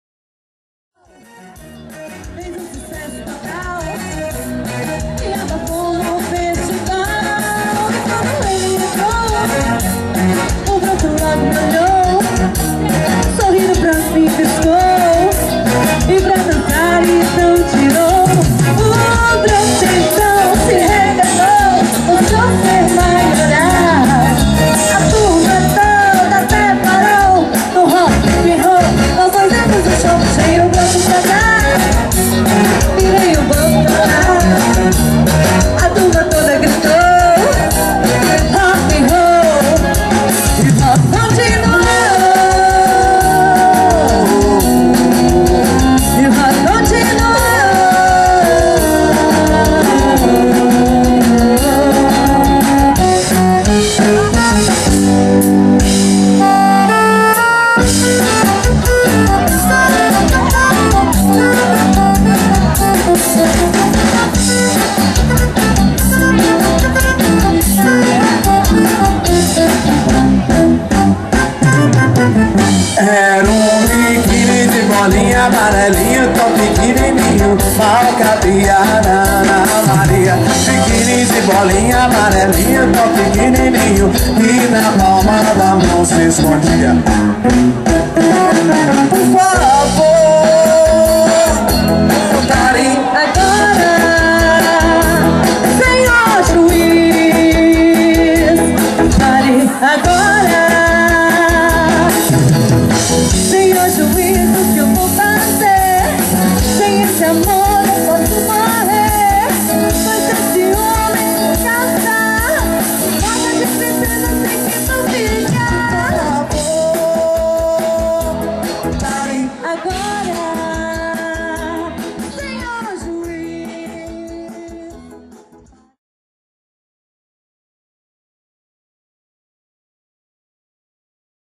Banda Baile BH anos 60 São Sebastião do maranhão MG
Gravado São Sebastião do Maranhão  Distrito de Mãe dos Homens MG XXV Festa da acormâe Banda Cia Dubhayle a mais animada de BH festas aniversários shows e eventos